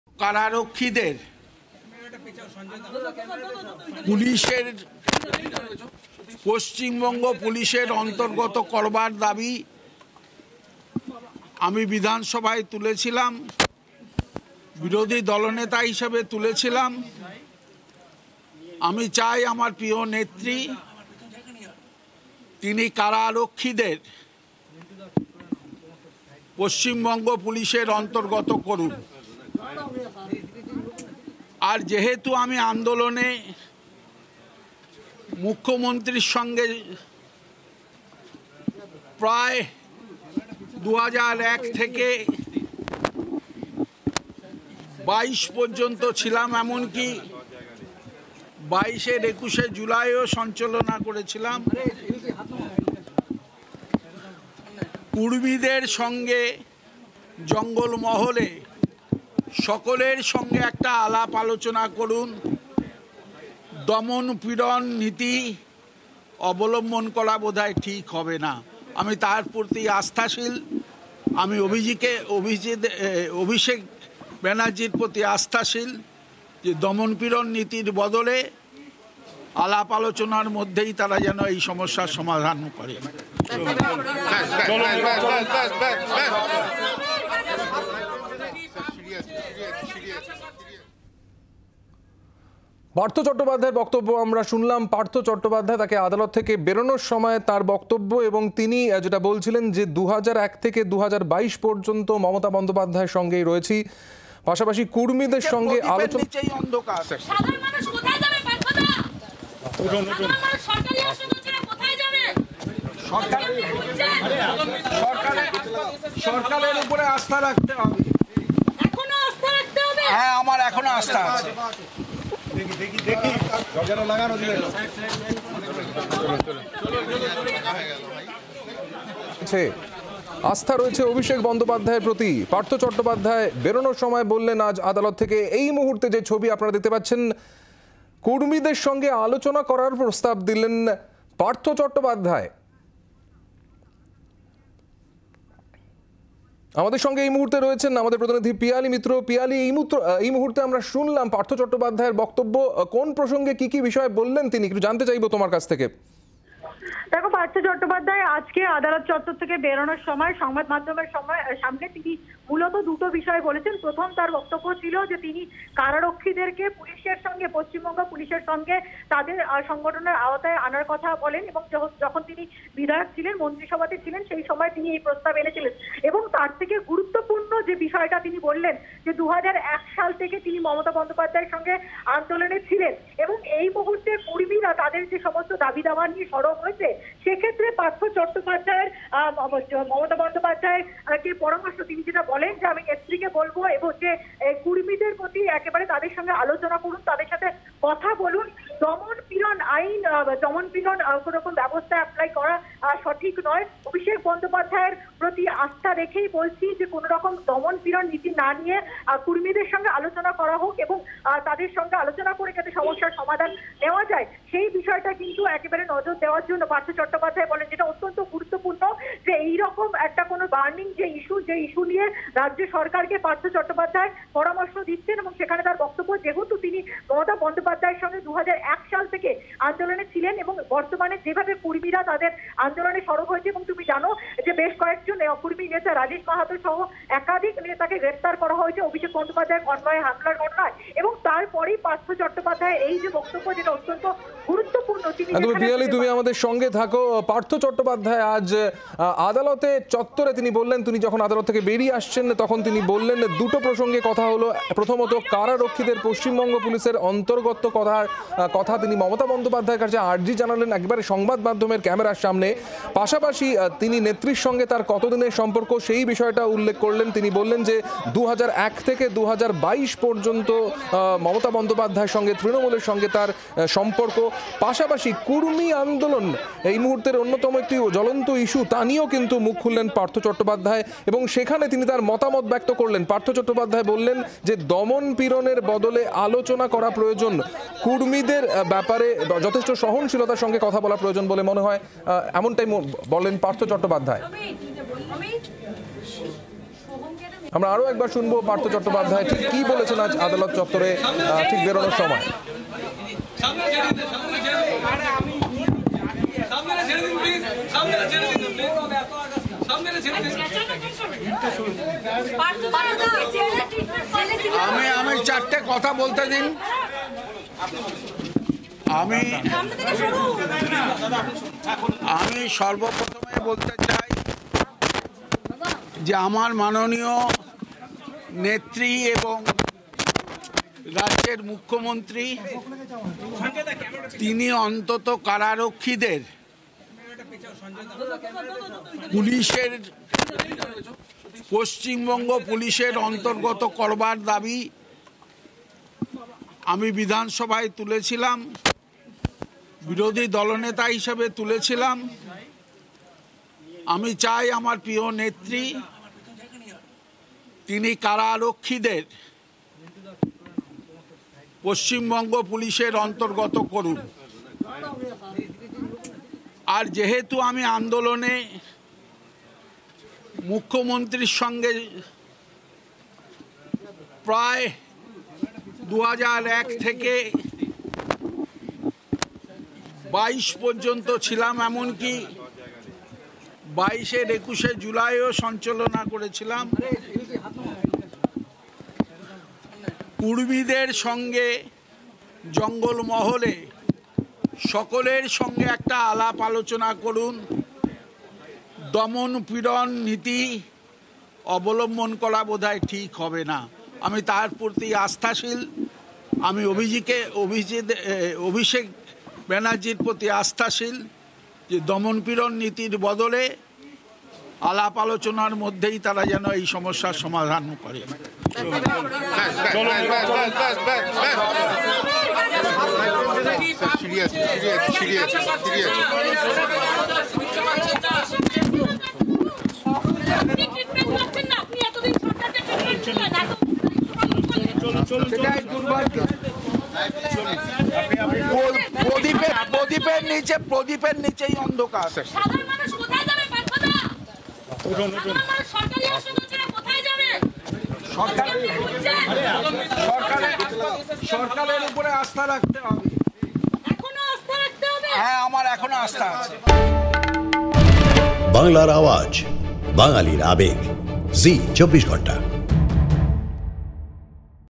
Partha Chatterjee: আদালতে দাঁড়িয়ে কুড়মি আন্দোলন নিয়ে মমতা ও অভিষেককে অনুরোধ পার্থর | Zee 24 Ghanta
Partha Chatterjee: আদালতে দাঁড়িয়ে  কুড়মি আন্দোলন নিয়ে মমতা ও অভিষেককে অনুরোধ করলেন পার্থ | Zee 24 Ghanta